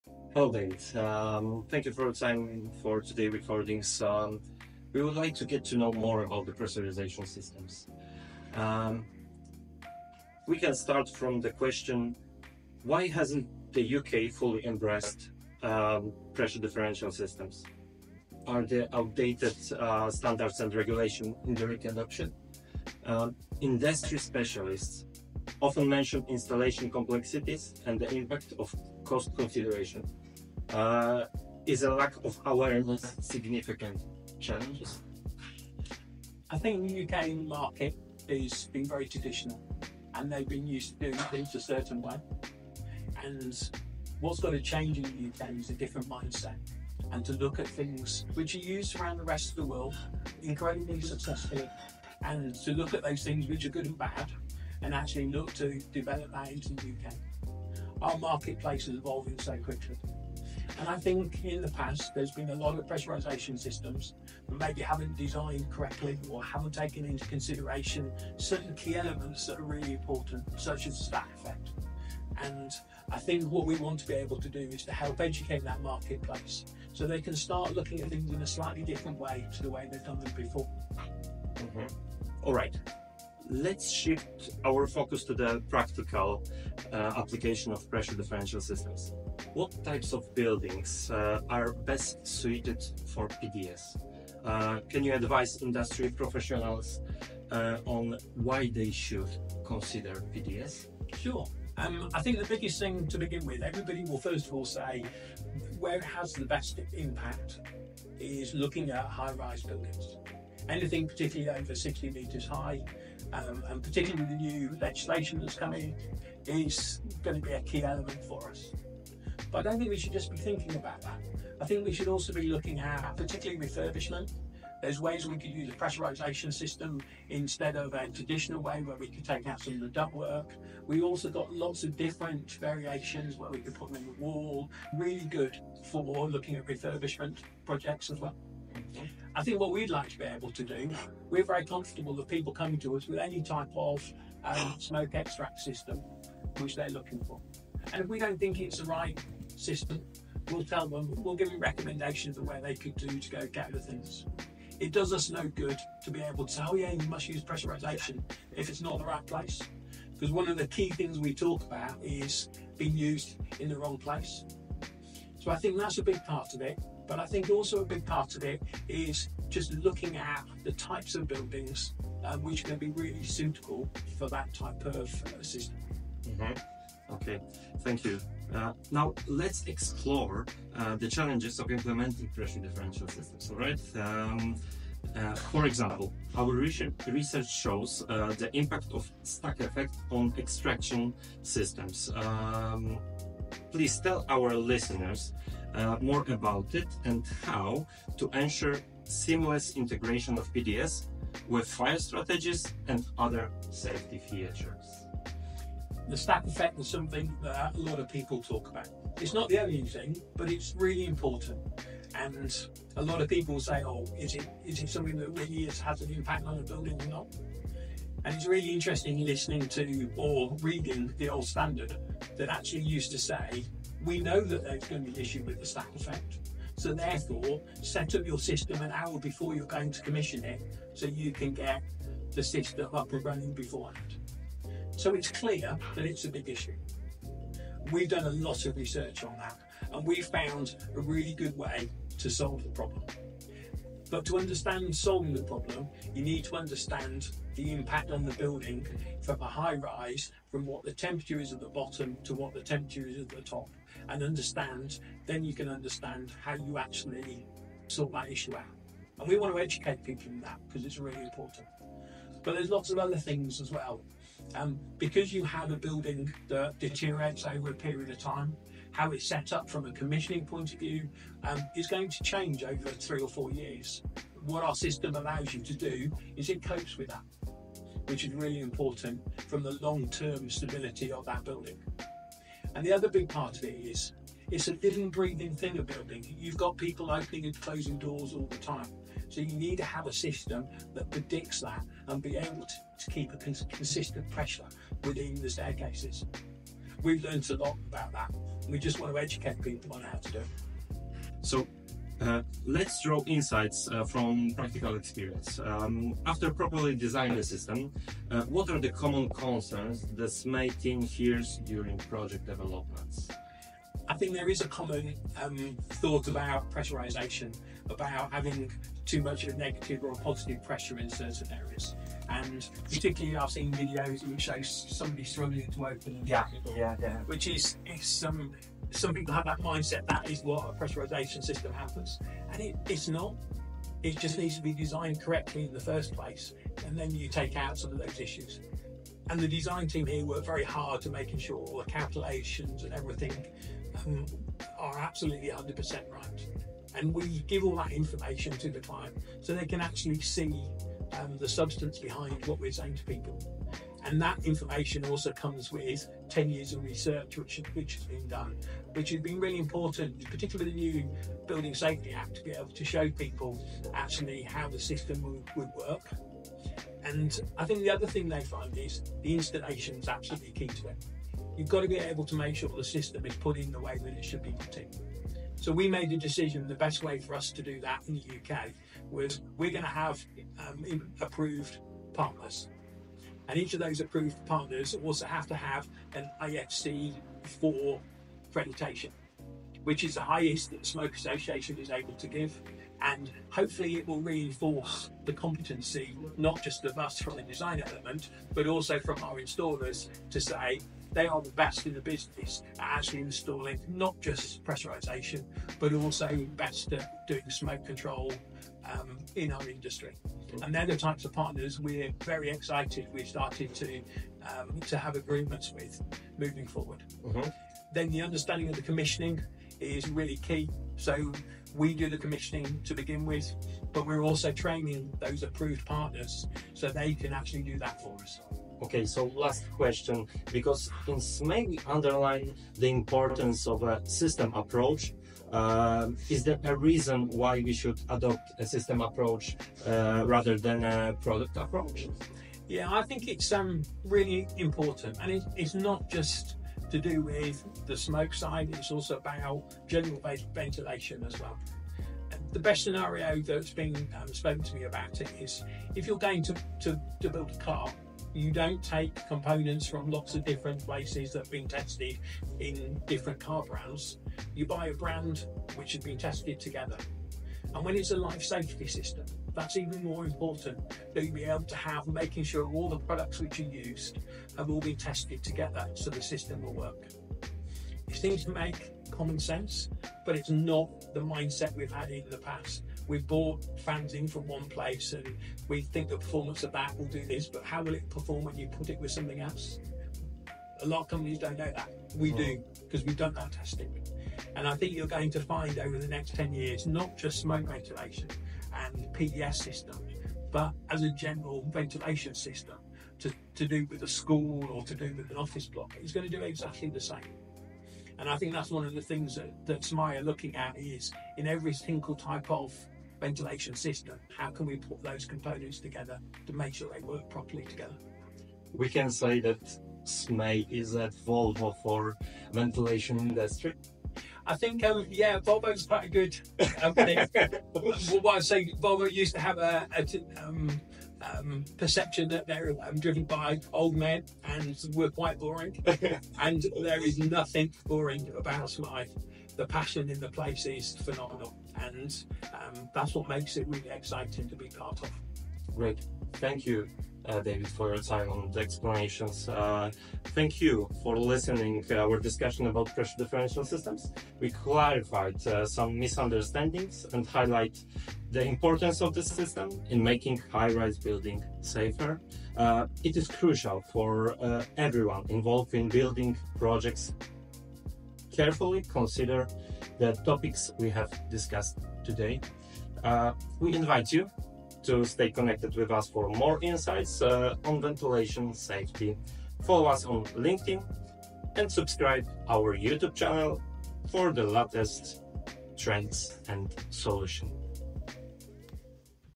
Debunking myths about smoke protection and pressure differential systems (PDS) in buildings
Would you like your team to upgrade their knowledge and tools on the pressurisation systems? Let's talk about it.

In our conversation with David Fitzpatrick, we delved into the intricacies of pressure differential systems, dispelling misconceptions and underlining their pivotal role in bolstering the safety of high-rise structures. These insights are of utmost importance for all stakeholders in the building projects arena to ponder upon.

David has been a leader and consultant in the ventilation and smoke industry for 40 years, making him a recognised expert. The SMAY team's expertise from more than 100+ pressurisation projects they develop gives him an outstanding perspective on solutions in the fire safety industry.

What we’ve discussed?

1. Why hasn't the UK fully embraced pressure differential systems?
2. When should industry professionals consider pressure differential systems?
3. What are the challenges of implementing pressure differential systems?
3. Why should we adopt a system, not product approach?

- Impact of PDS on high–rise buildings and conclusion of the study